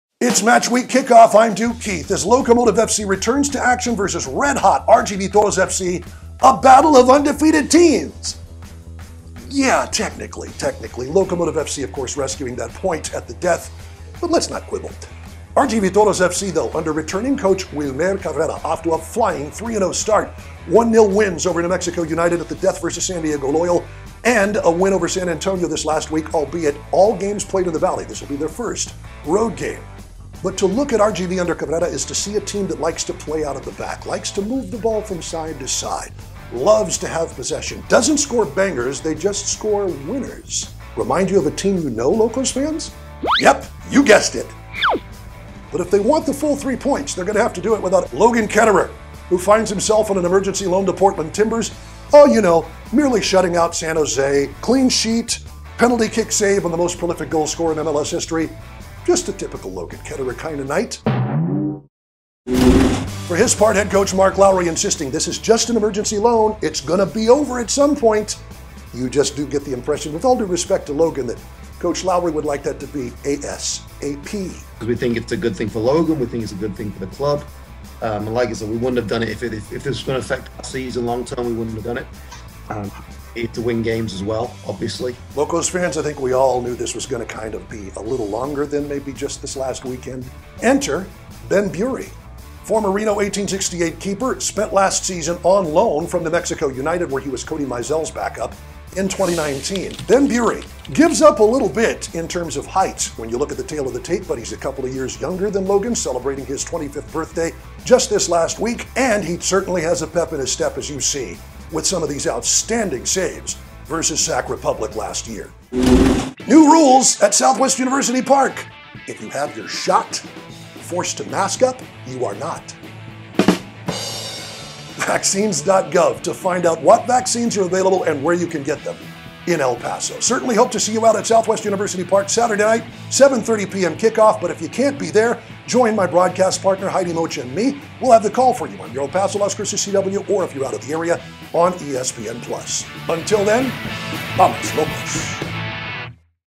WATCH: Locomotive Match Week Kickoff!
WATCH: LOTS of Locomotive news leading in to tomorrow's big game!
✅ A look at RGV Toros FC
✅ Wassup with Logan Ketterer?
✅ Footage of Ben Beaury
✅ I spit mad rhymes on LosLocos new mask policy!
(Well, okay, maybe not so mad. 🤷🏽‍♂️🤣)
It's MatchWeekKickoff!